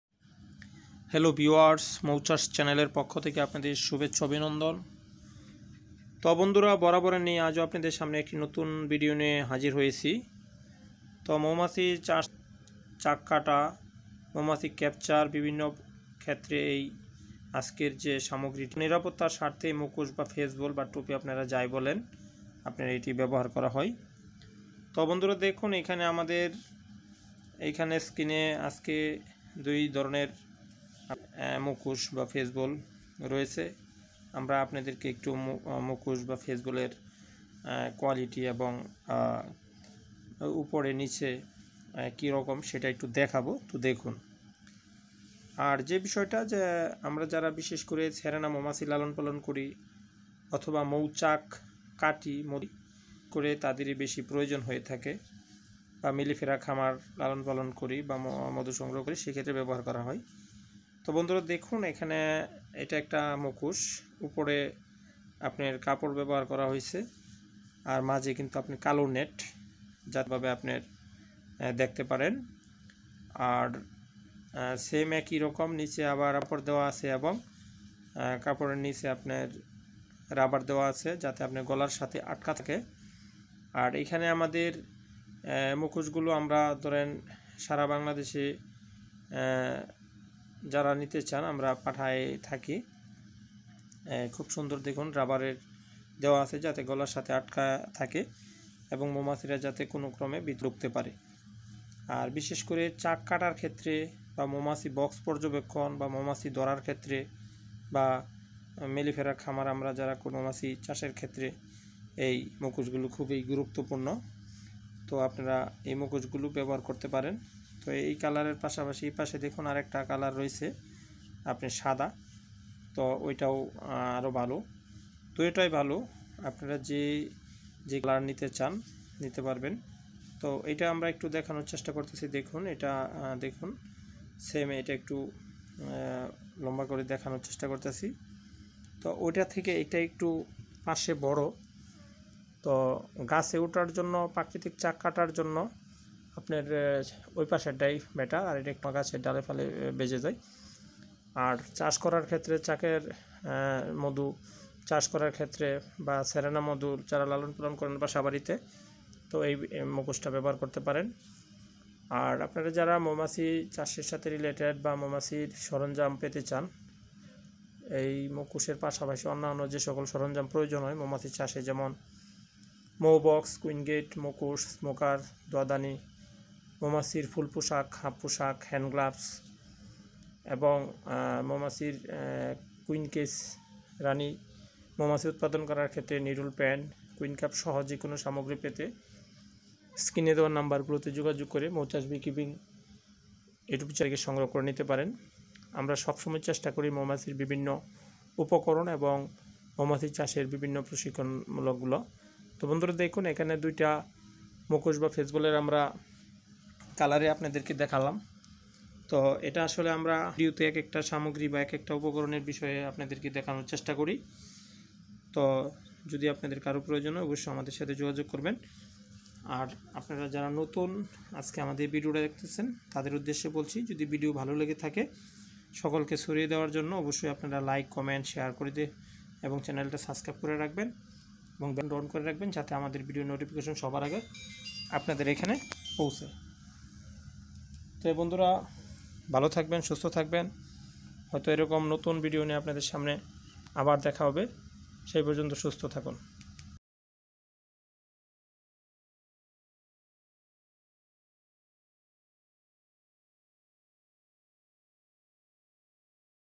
মুকোশ বা টুপি বা ফেইসবল মৌমাছি চাষের ও মৌ চাক কাটার গুরুত্বপূর্ণ মৌ উপকরণ সামগ্রী।
মৌমাছির চাষ পদ্ধতি
মৌ পালন পদ্ধতি

মৌমাছির চাষের সকল যন্ত্রপাতি পেতে

০১৯৬৩১১৬৫৩৯,০১৭৪১১৭০২২৩ ইমু